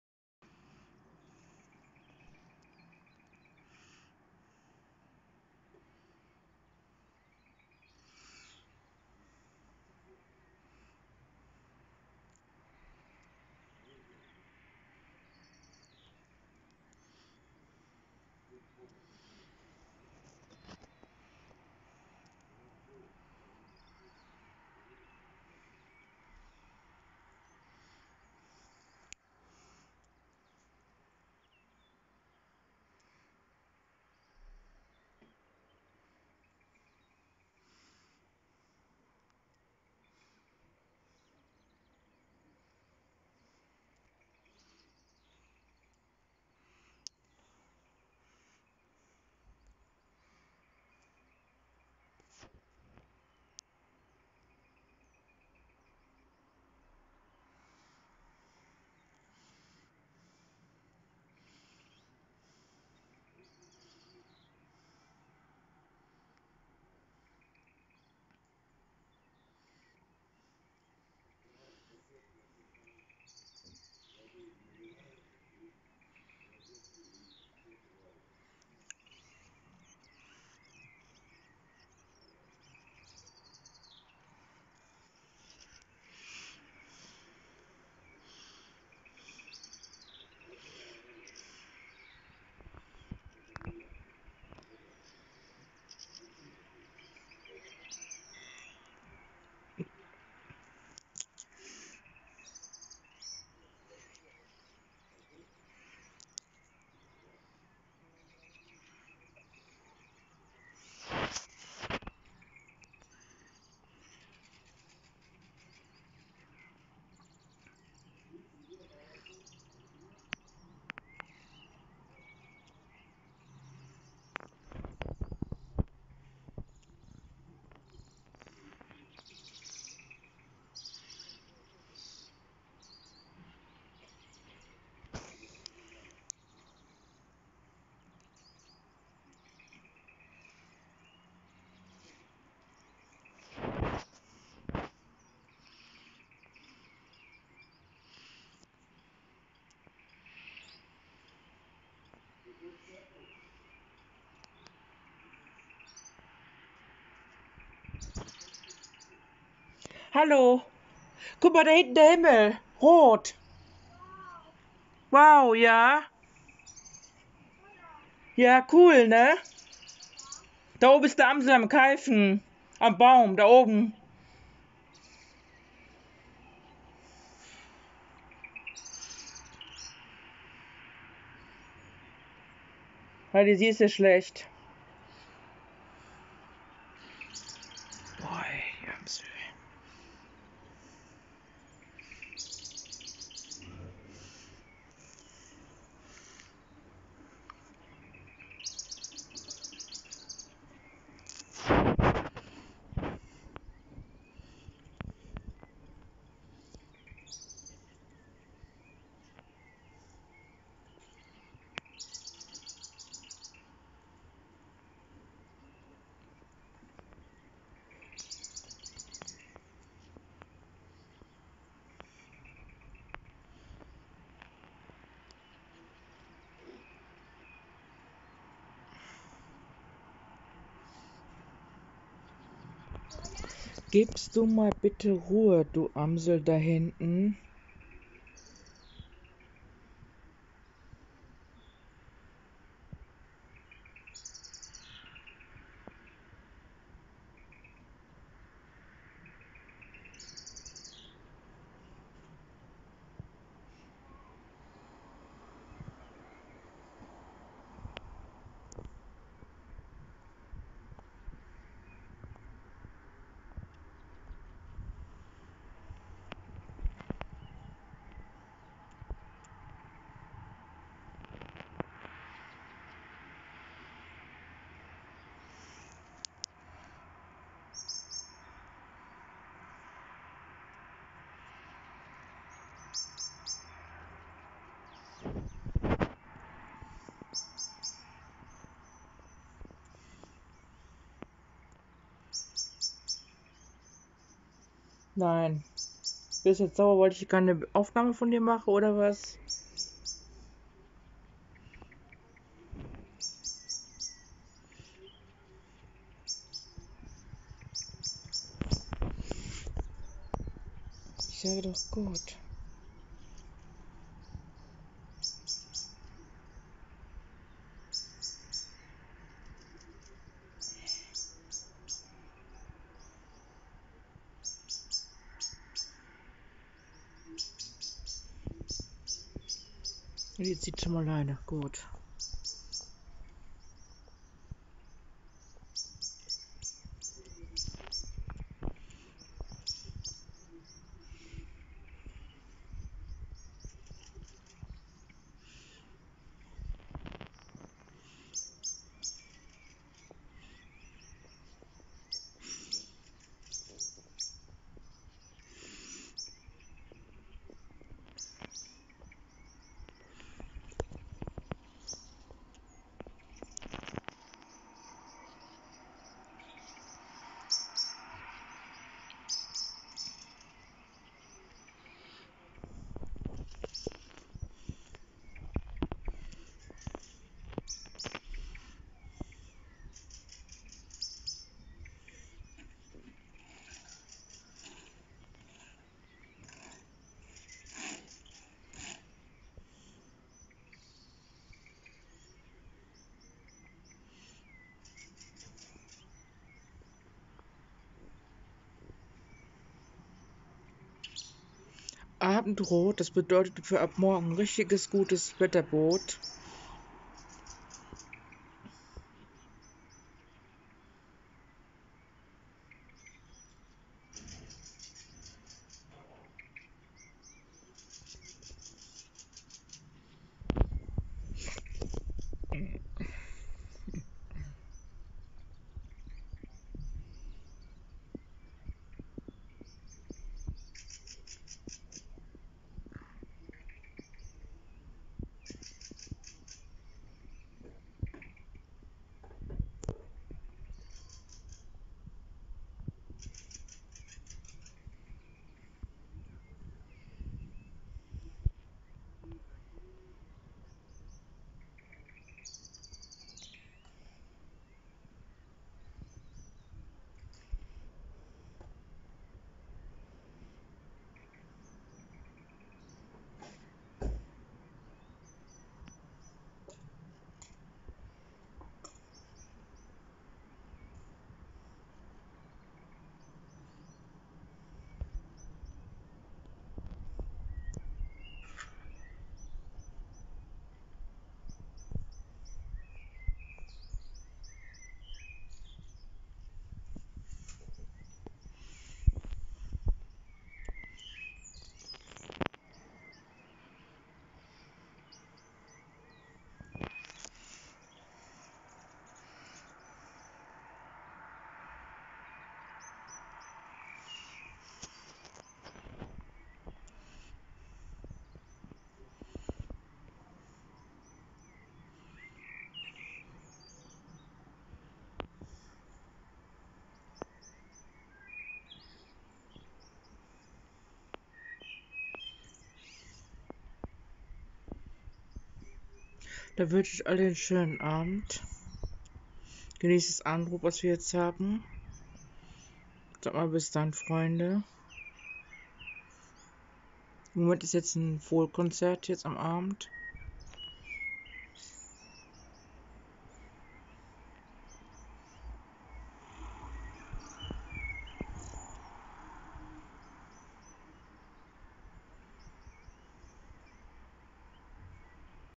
24.4.2021. WOW, SCHÖNER FEUERROTER SAHARA HIMMEL AM ABEND IN MESCHEDE!😍❤️🌅🌤️
das nenne ich Mal eine Glücksaufnahme. ich wünsche euch allen einen schönen Abend.😊